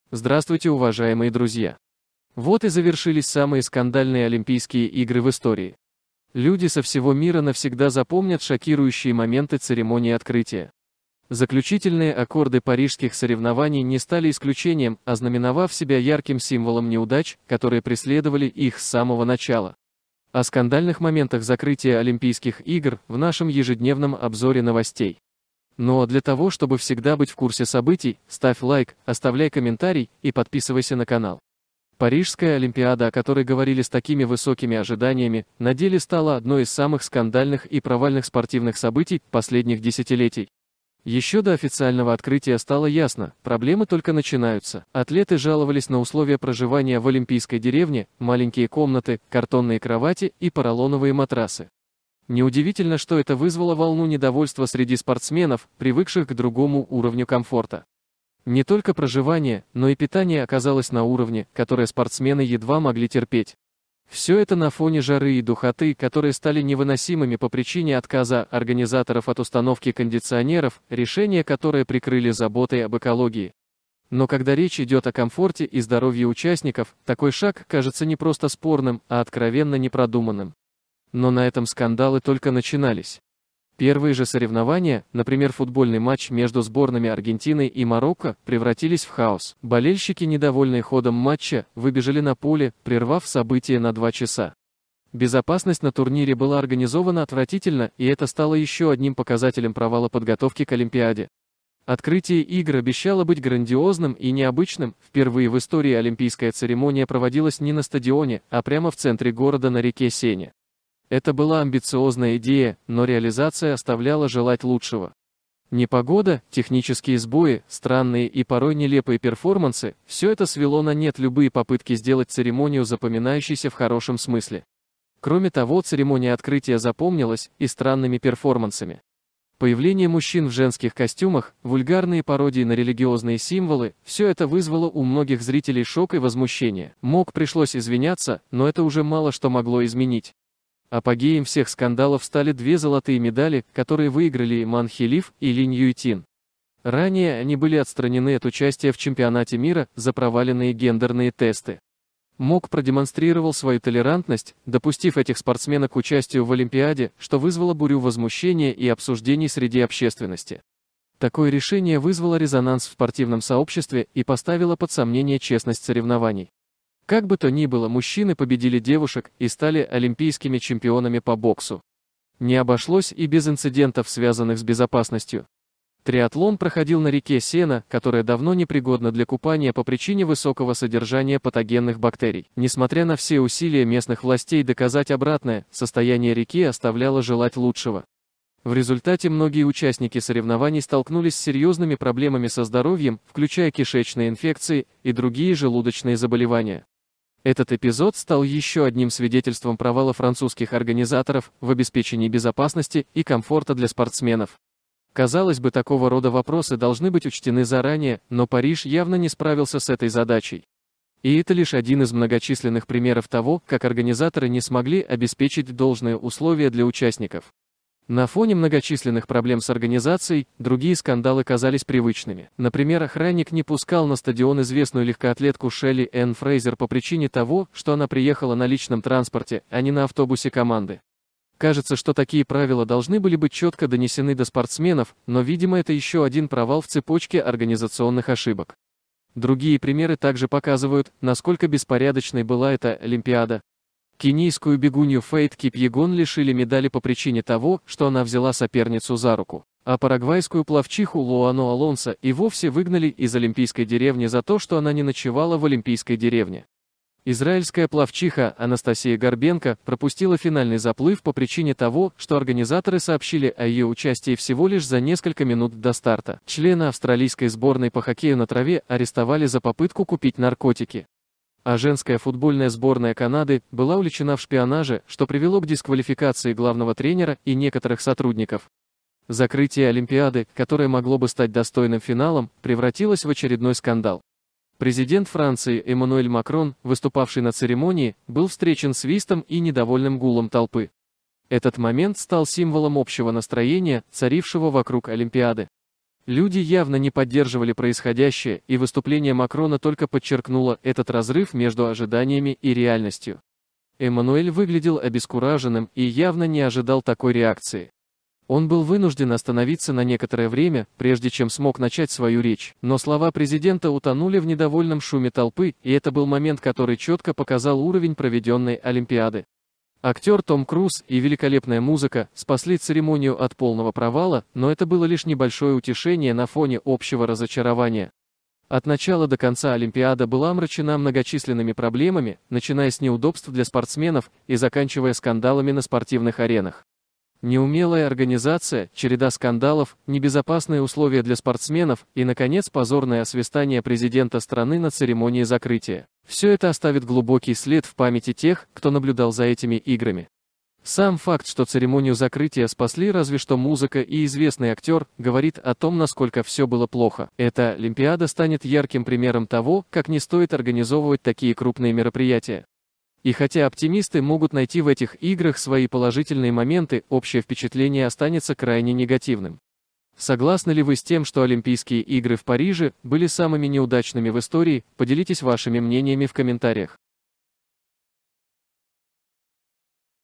ЦЕРЕМОНИЯ ЗАКРЫТИЯ ЧЕЛОВЕЧЕСТВА! МОК и Бах Не Ожидали Такого Финала от Французов на Завершении ОИ!
Здравствуйте уважаемые друзья. Вот и завершились самые скандальные Олимпийские игры в истории. Люди со всего мира навсегда запомнят шокирующие моменты церемонии открытия. Заключительные аккорды парижских соревнований не стали исключением, ознаменовав себя ярким символом неудач, которые преследовали их с самого начала.

Закрытие Олимпиады, которое могло бы стать достойным финалом, превратилось в очередной скандал. Президент Франции Эммануэль Макрон, выступавший на церемонии, был встречен свистом и недовольным гулом толпы. Этот момент стал символом общего настроения, царившего вокруг Олимпиады. Люди явно не поддерживали происходящее, и выступление Макрона только подчеркнуло этот разрыв между ожиданиями и реальностью.

Эммануэль выглядел обескураженным и явно не ожидал такой реакции. Он был вынужден остановиться на некоторое время, прежде чем смог начать свою речь. Но слова президента утонули в недовольном шуме толпы, и это был момент который чётко показал уровень проведенной Олимпиады. Актёр Том Круз и великолепная музыка спасли церемонию от полного провала, но это было лишь небольшое утешение на фоне общего разочарования.

О скандальных моментах закрытия Олимпийских игр, в нашем ежедневном обзоре новостей.